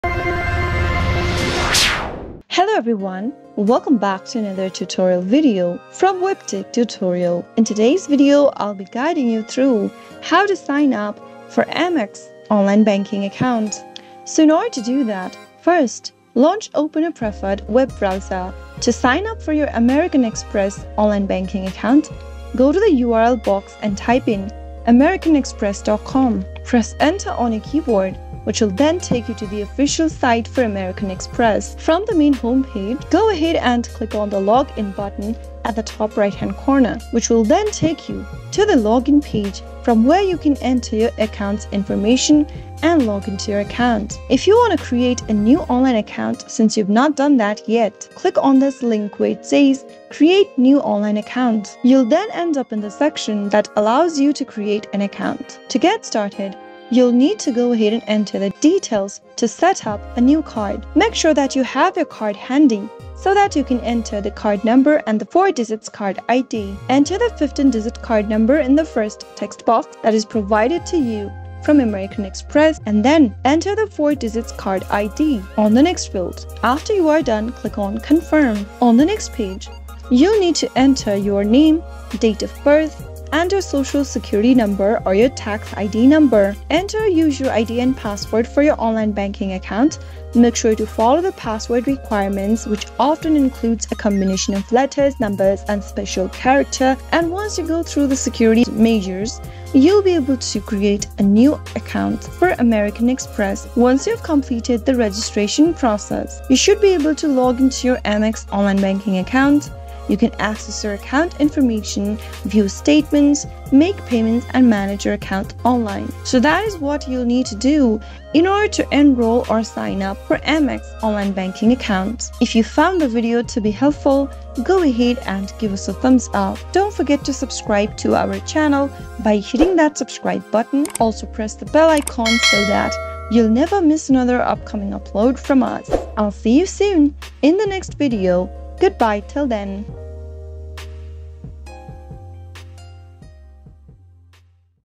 How to Sign Up American Express Online Banking Account Online 2023? Make/Open Amex Account
American Express online banking is a convenient and easy way to manage your account. You can check your balance, make payments, view statements, and more. In this video, I'll show you how to sign up for American Express online banking in 2023.